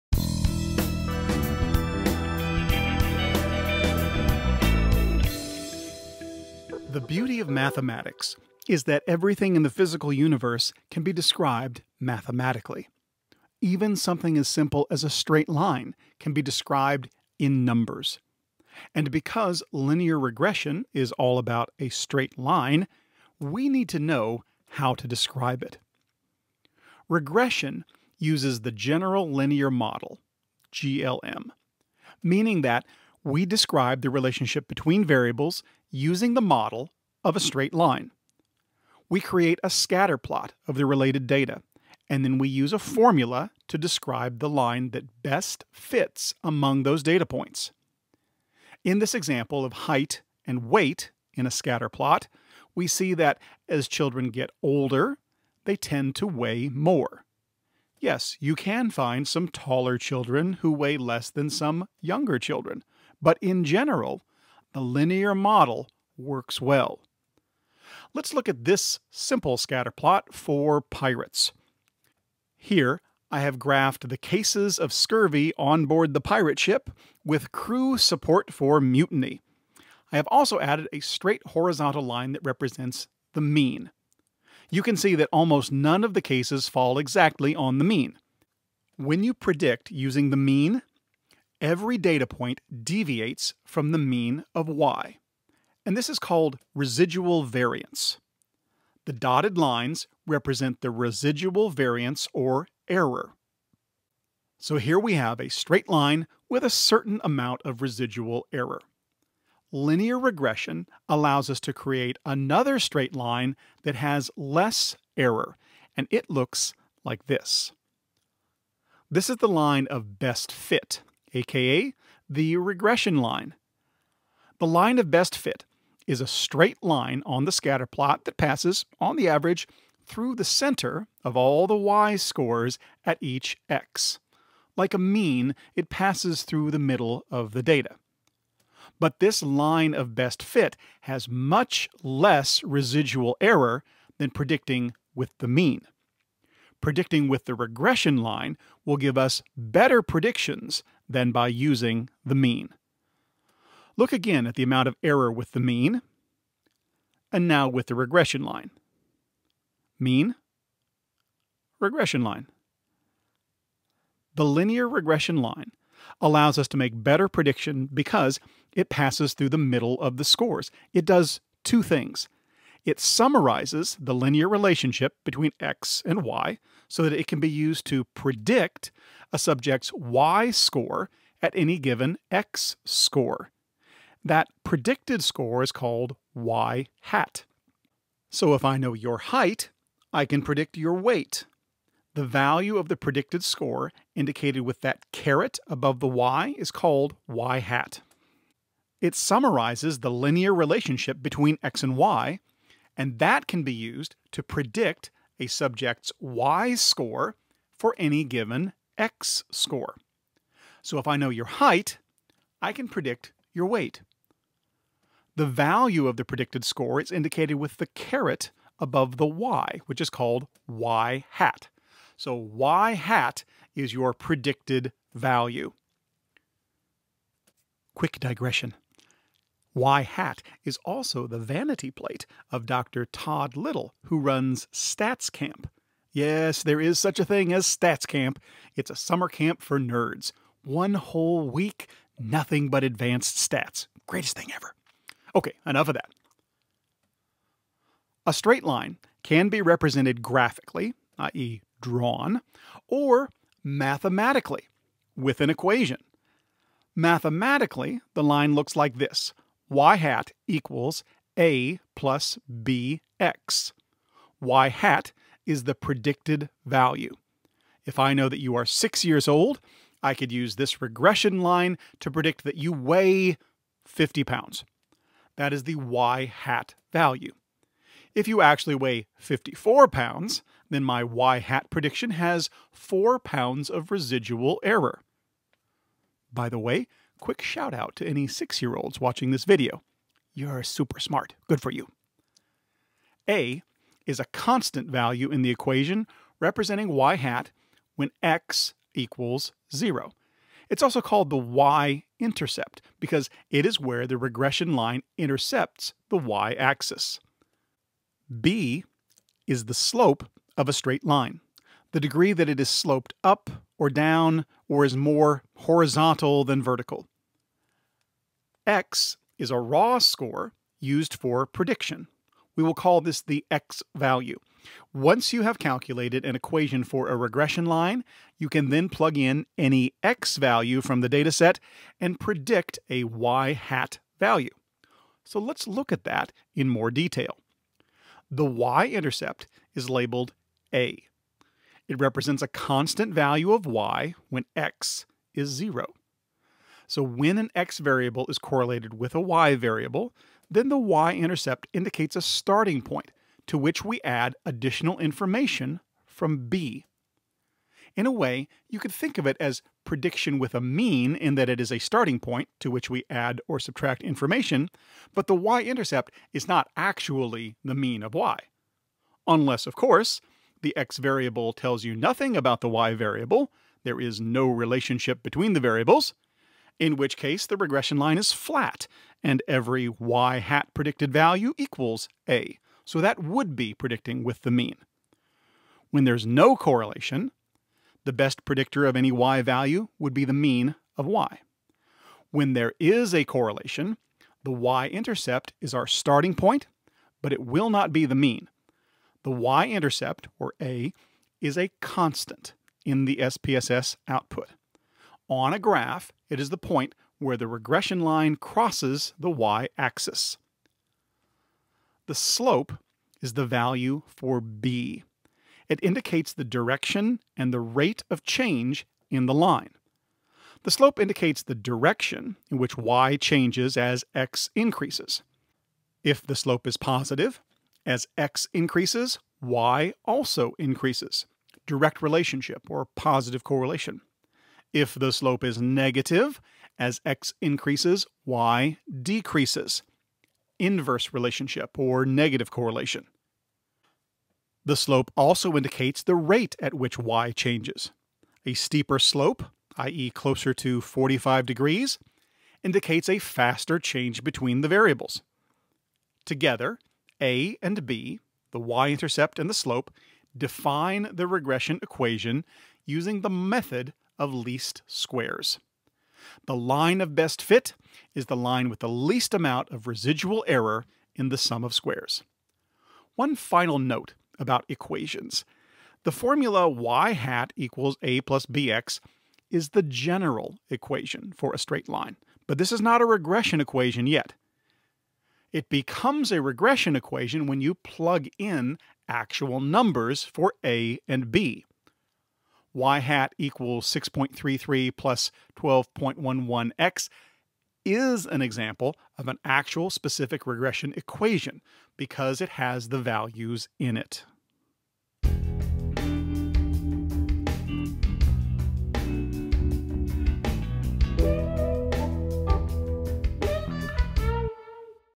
Foundational Regression – The Linear Regression Line (14-2)
The beauty of mathematics is that everything in the physical universe can be described mathematically. Even something as simple as a straight line can be described with numbers. Linear regression uses the general linear model (GLM); meaning that we describe the relationship between variables using the model of a straight line. We create a scatterplot of the related data then use a formula to describe the line that fits best among those data points.

This video teaches the following concepts and techniques:
Introduction to the regression line

Link to a Google Drive folder with all of the files that I use in the videos including the regression datasets. As I add new files, they will appear here, as well.